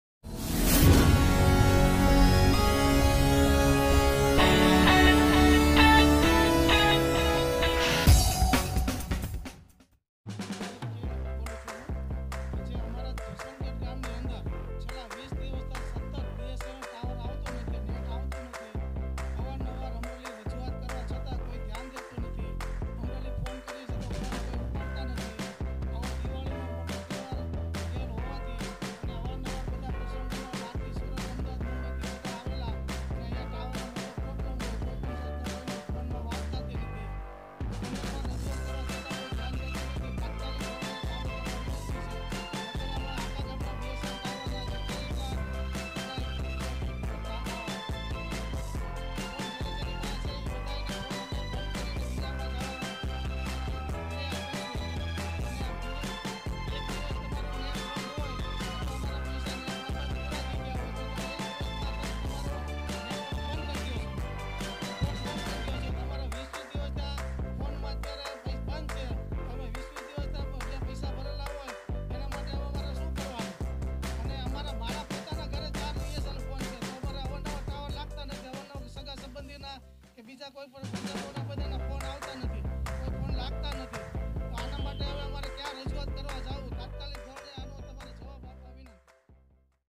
SPEEDNEWS17 અમરેલી ના જસવંતગઢ ગામમાં બીએસએનએલ (BSNL) ના નેટવર્ક થી લોકો પરેશાન..
બીએસએનએલ (BSNL) ના નેટવર્ક થી લોકો પરેશાન..

અમરેલી ના જસવંતગઢ ગામ માં છેલ્લા વીસ  દિવસ થી  બીએસએનએલ ટાવરબંધ.  દિવાળી જેવા  તહેવારો મા પણ બંધ રહેતા લોકો માં રોષ  જસવંતગઢ ગામ ના ઉપ સરપંચ જે.બી.દેસાઈ.  દ્વારા ઉચ્ચ કક્ષા એ રજુઆત કરી.

બ્યુરો રીપોર્ટ સુરજ પરમાર ચિતલ શહેર